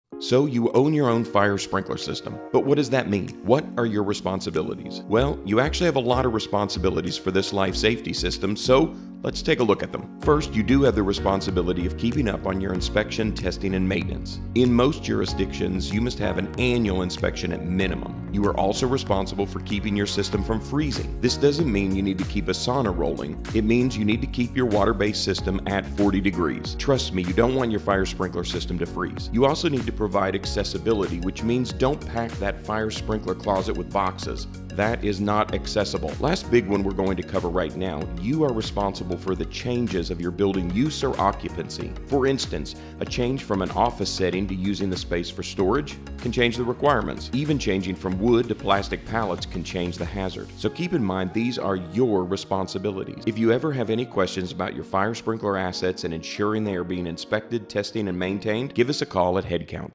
Owning a Fire Sprinkler System
We'll look at some of the requirements for owning a fire sprinkler system - based on NFPA 25.